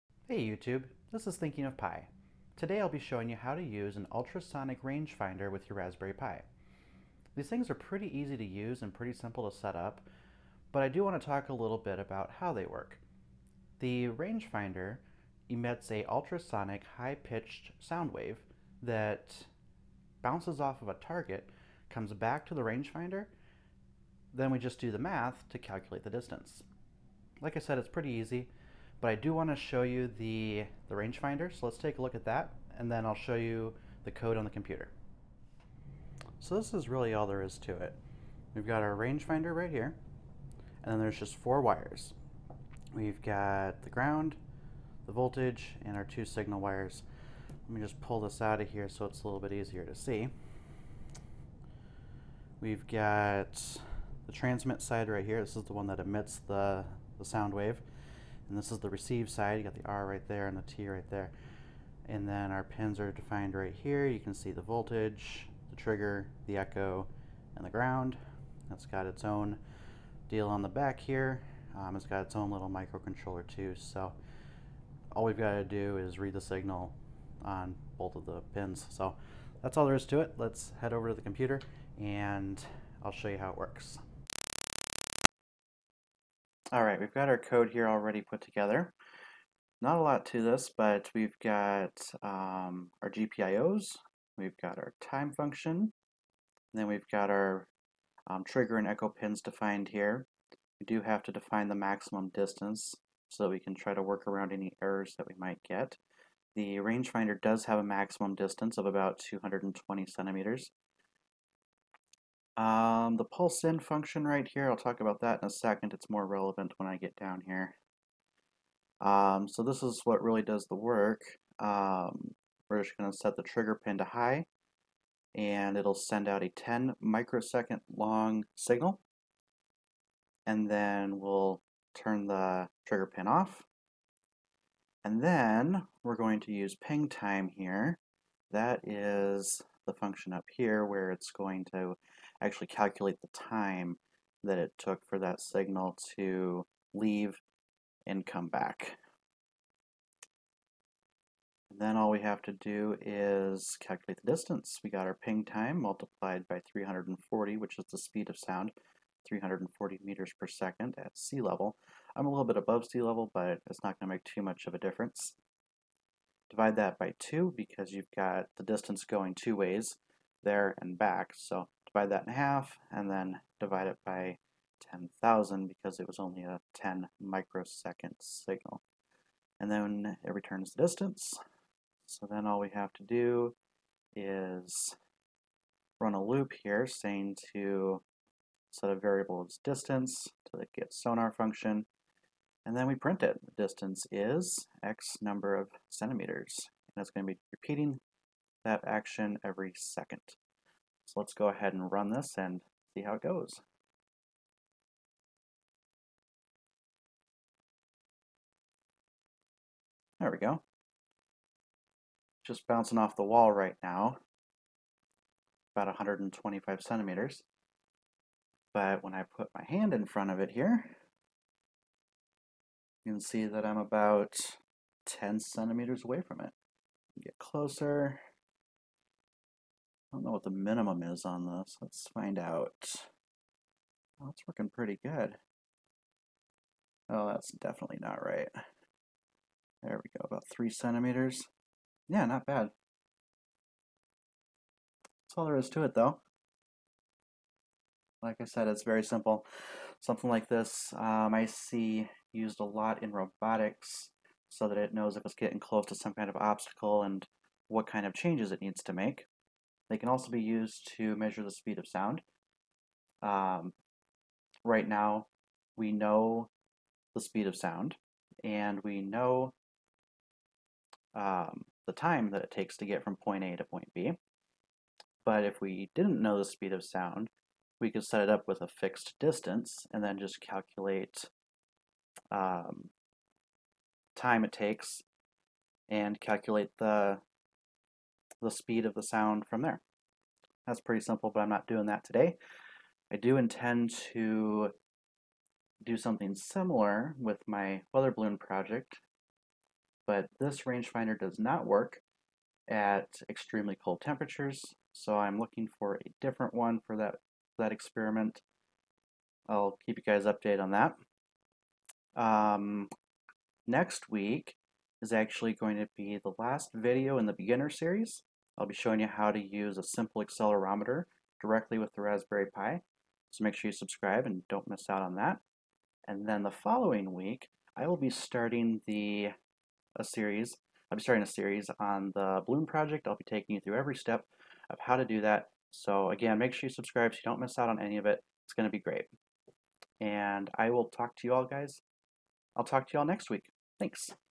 Ultrasonic Range Finder | Raspberry Pi 3 B+ | Projects for Beginners
In this video I show how to use the HC-SR04 ultrasonic range finder with the Raspberry Pi